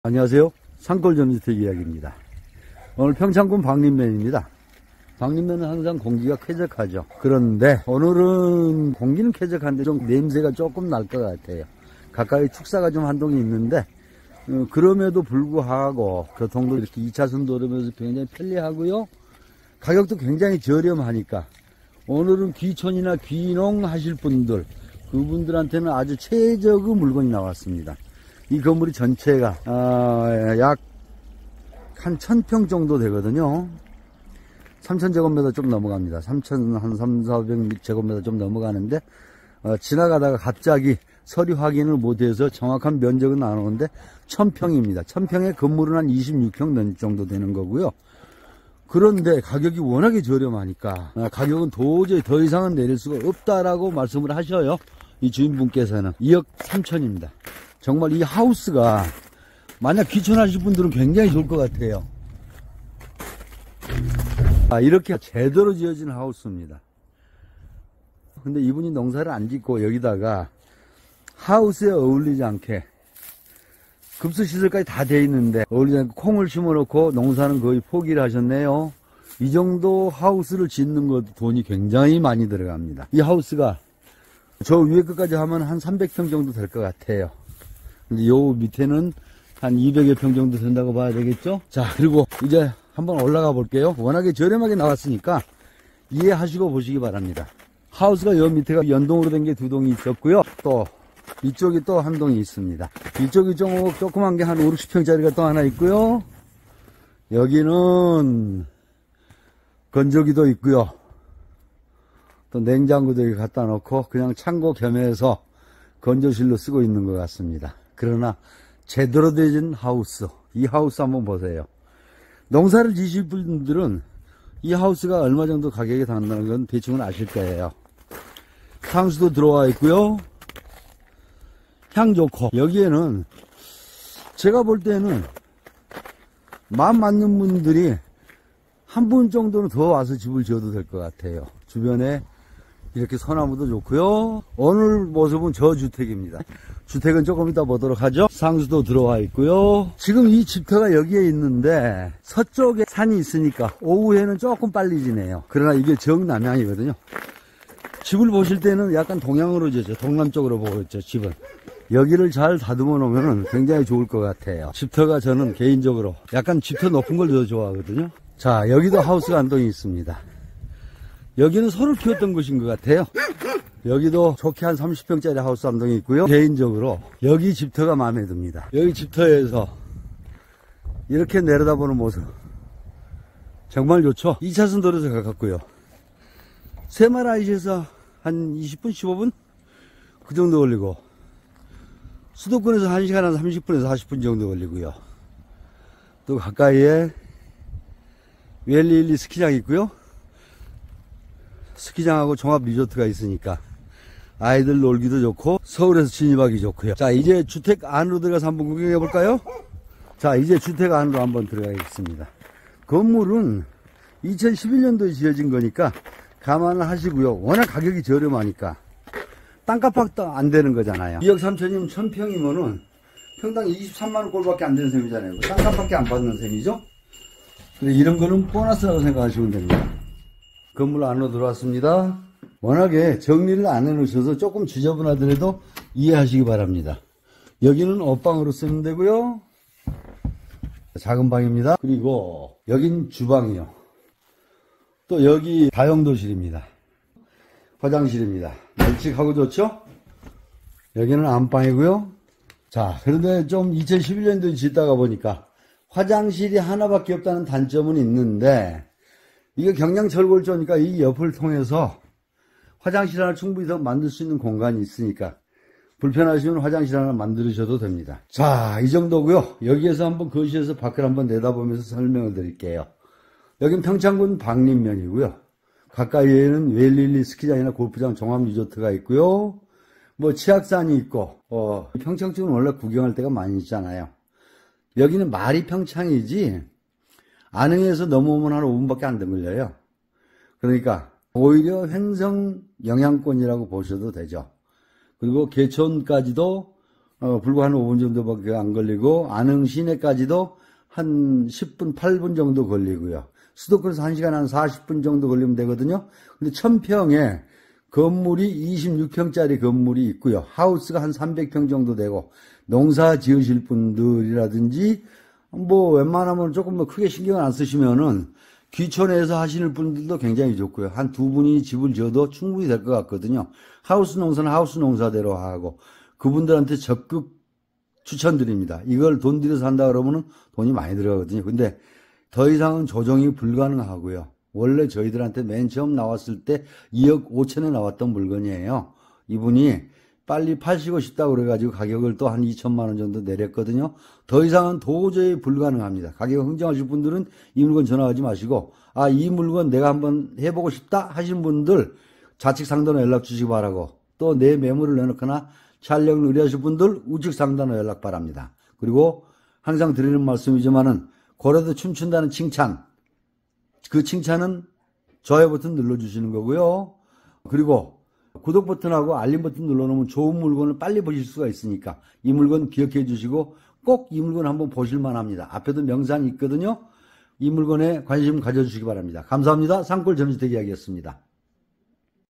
최저가 귀농귀촌인 1169평 2억원대 평창 방림 준고냉지평창 전원주택 강원도전원주택매매 평창전원주택 평창주말주택 [매번1평-2051]
♡ 매물번호: 1평-2051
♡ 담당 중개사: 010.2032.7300
♡물건소재 : 평창군 방림면
♡토지면적 : 3865㎡  /1169평
♡건물면적 : 85.32㎡  / 25.8평
♡건축구조 : 경량철골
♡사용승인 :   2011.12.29
♡건물구성 : 방2 화장실1 다용도실 특수비닐하우스 300여평
♡용도지역 : 계획관리
♡매매가격 : 2.3억원 조절불가
♡입주시기 : 협의
♡주차대수 : 3
♡관리비용 : 별도의 관리비 없음
♡주택방향 : 거실기준 남향
♡도로관계 : 2차선접
♡통신환경 : 전화 인터넷 휴대폰통신 원할
♡식수 ; 대공 광역상수도
♡난방 : 등유 화목보일러
♡텃밭면적 : 하우스300여평 포함 900평
♡이용현황 : 전원주택
♡주변환경 : 인근 축사1동 새말ic20분  2차선접 교통편리 수도권1시간40분
♡주변명소 : 웰리힐리스키 골프 종합리조트  치악산20분  10분이내 뇌운계곡
                         흥정계곡
♡추천용도 : 귀농귀촌인 저렴한 가격 하우스 농사 1동추가 건축가능
♡기타사항 : 특수잘지은 비닐하우스300평 귀촌과 수입을 동시에
♡지번공개 : 계약완료전까지 전화상담 방문고객에게 공개 드립니다.

시골 생활 30여년의 경험담을 우리  가족님들과 공유 하고자  시작 하였습니다.
많은 구독 바랍니다

산골전원주택이야기 공인중개사사무소
대표 : 박병규 (강원 가3724-105)
강원도 횡성군 서원면 경강로 797